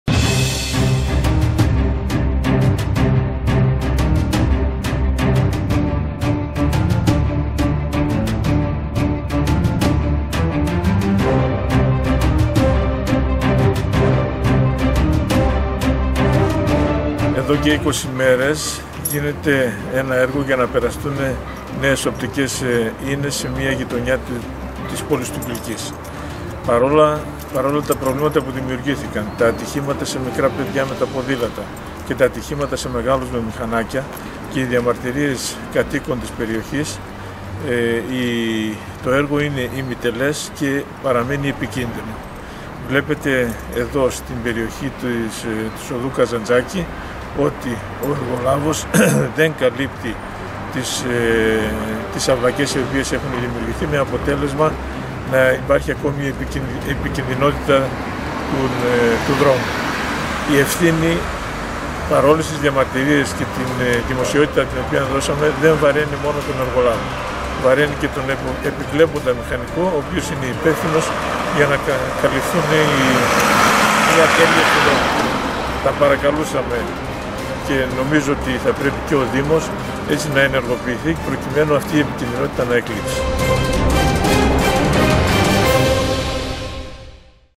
Επικίνδυνος δρόμος με ανοιχτές …αυλακιές στο Κιλκίς
Σάββατο 23 Νοεμβρίου 2019. Επικίνδυνος δρόμος (Οδός Καζαντζάκη ) με ανοιχτές …αυλακιές στο Κιλκίς.